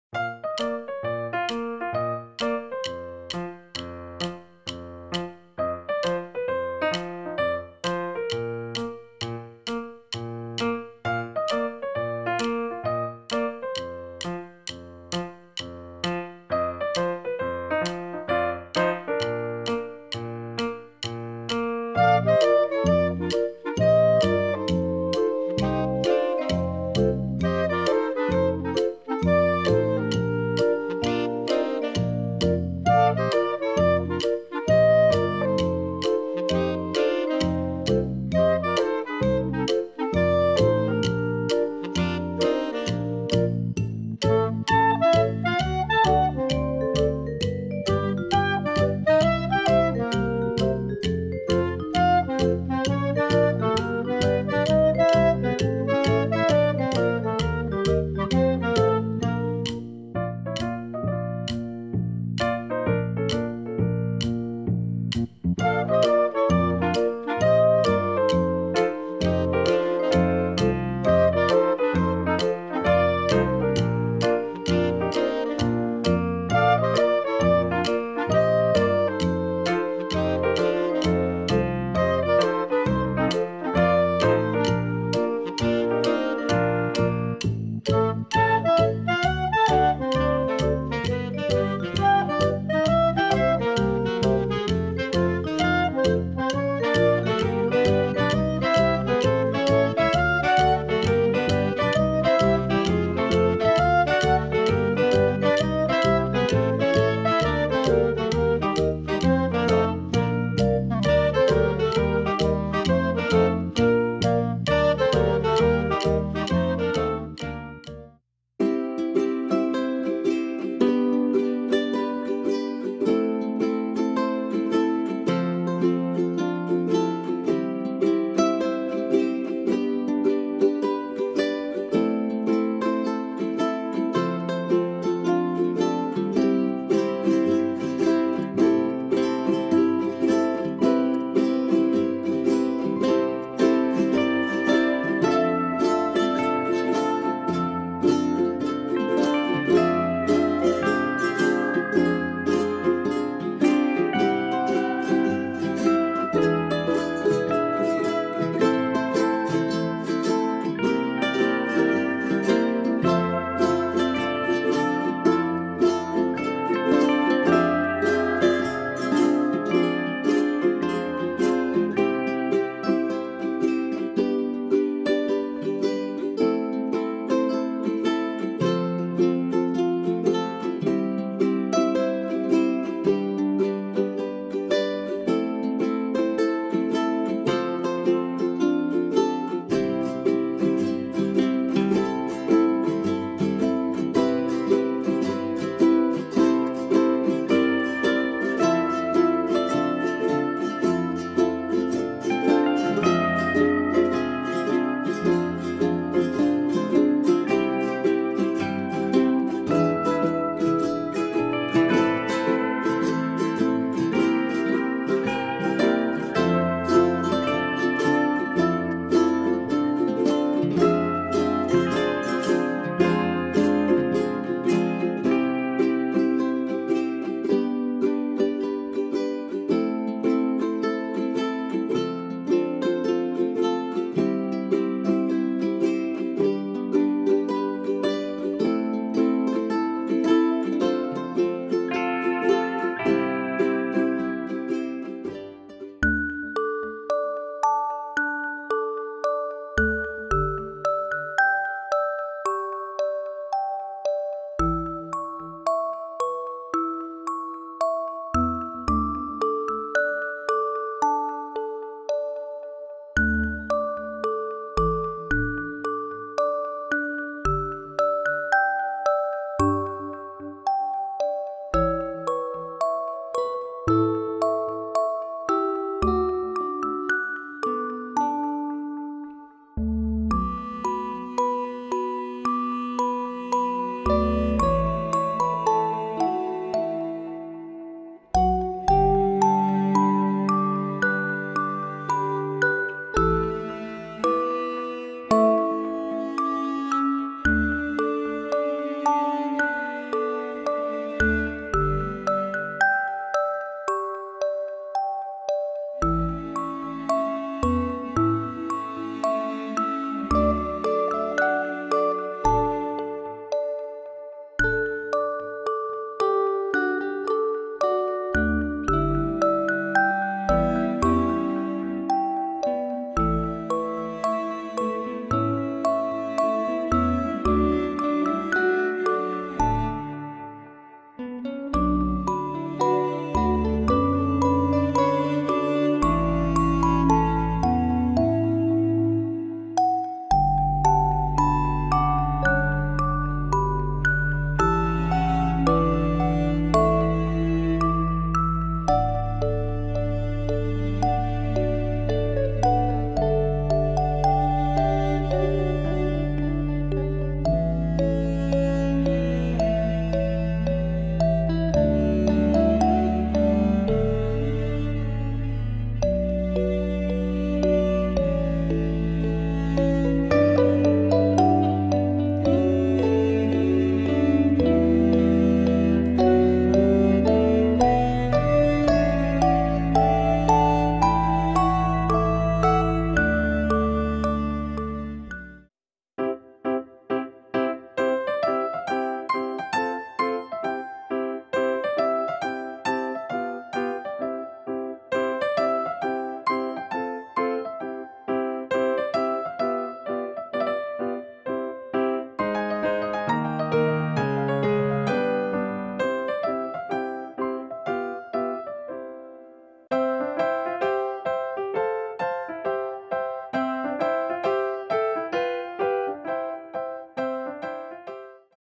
Cute Panda Friends Colored sand painting how to painting with sand
Colored sand painting Cute Panda Friends how to painting with sand | sand art for kids

cute, art for kids, step by step, easy for kids, Kaily Drawing Box